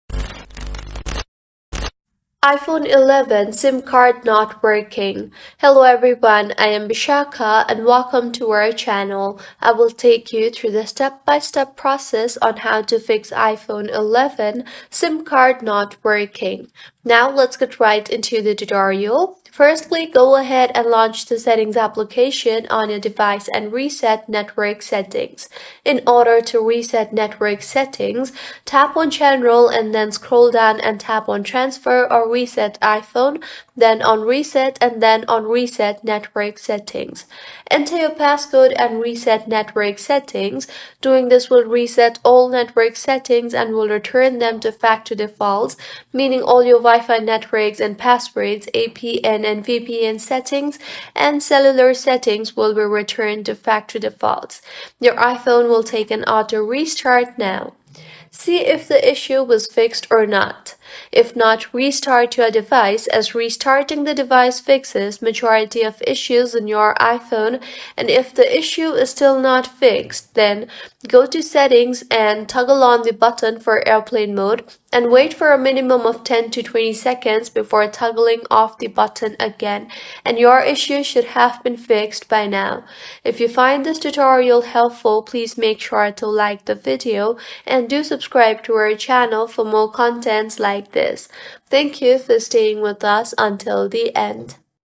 iPhone 11 SIM Card Not Working? No Service, No SIM Card, Invalid SIM, Stuck on Searching FIXED!
Hey there! Are you experiencing issues with your iPhone 11 SIM card like "No Service," "No SIM Card," "Invalid SIM," or being "Stuck on Searching"? Don't worry, I've got you covered! In this video, I'll show you how to fix these common SIM card problems step by step. Whether you're encountering these issues for the first time or have been struggling with them for a while, follow along as I guide you through troubleshooting and fixing your iPhone 11 SIM card issues. Let's get your iPhone back up and running smoothly!

Timestamps:

0:00 - Introduction: Common iPhone 11 SIM Card Issues
0:10 - Troubleshooting "No Service" and "No SIM Card" Errors
0:20 - Fixing "Invalid SIM" and "Stuck on Searching" Problems